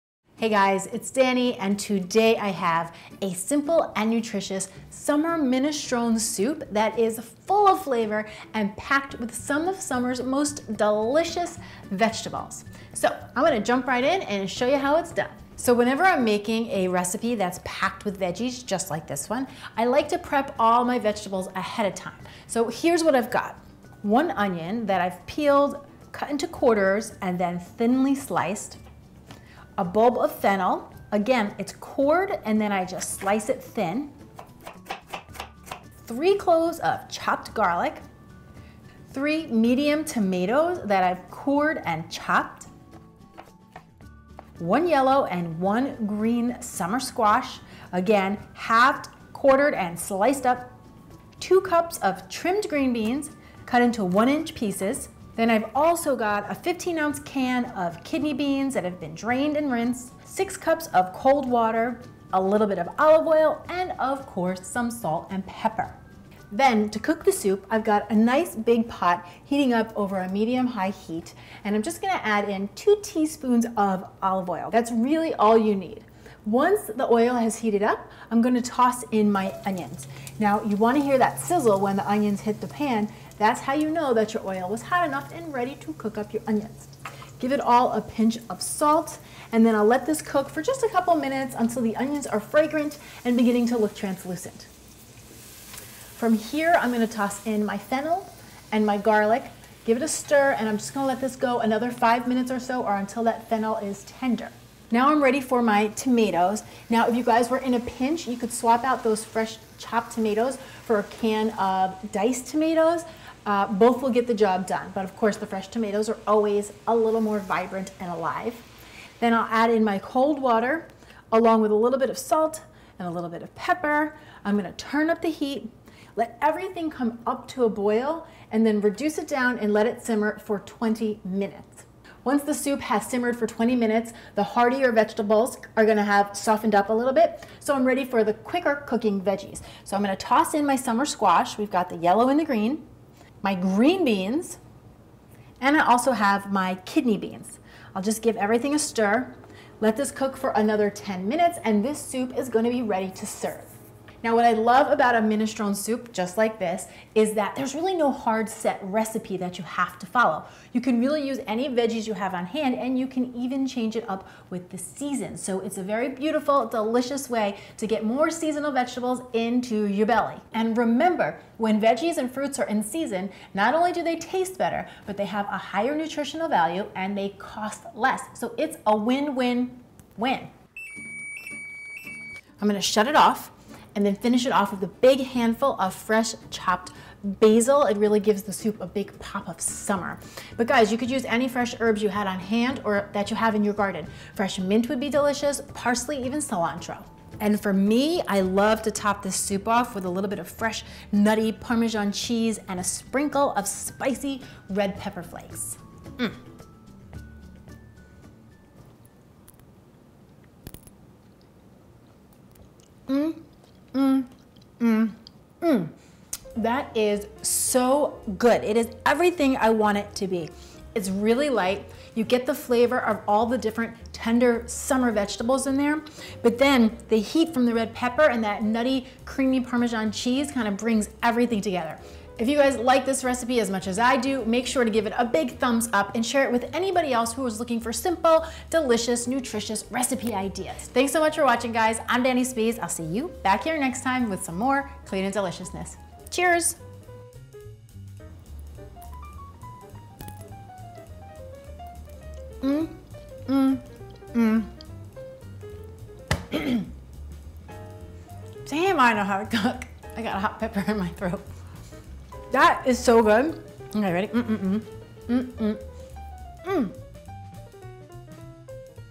Summer Minestrone Soup | Clean & Delicious
This Summer Minestrone Soup is packed with the seasons most abundant veggies; including summer squash, green beans and tomatoes.

HELPFUL KITCHEN TOOLS:
Santoku Knife
Glass Measuring Cups
Le Cruset 5 1/2 Quart Pot

SUMMER MINESTRONE SOUP RECIPE

1 onion, cut into quarters and thinly sliced
1 bulb of fennel, cored and sliced
3 gloves of garlic, minced
6 cups of cold water
2 large tomatoes, cored and cut into chunks (I did not bother to peel or seed them)
1 yellow squash, quartered and sliced
1 green squash, quartered and sliced
1 15 oz. can of kidney beans, rinsed and drained
2 cups of green beans
2 tsp of olive oil
Salt and pepper to taste
handful of fresh chopped basil

Directions:
Heat olive oil in a large pot over medium high heat.  Add onion and a fat pinch of salt and saute until the onions begin to brown around the edges, about 5 minutes.  Stir in the fennel and garlic and saute another 5 minutes or so, or until the fennel is tender.

Add tomatoes and about 6 cups of cold water.  Give it another hit of salt and pepper here, then crank that baby to high and bring to a boil.  Once boiling, reduce the heat and simmer for about 20 minutes.
Add  squash, greenbeans, and kidney beans.  Simmer until veggies are tender, about 10 minutes.

Stir in the fresh basil and serve with fresh grated parmesan and a pinch of hot pepper flakes (optional). Enjoy!

Calories per 1/6th of recipe: 125; Total Fat: 2.2g; Saturated Fat: 0.3g; Cholesterol: 22.8mg; Sodium: 140mg; Carbohydrate: 22.8g; Dietary Fiber: 7g; Sugars: 3.2g; Protein: 6.3g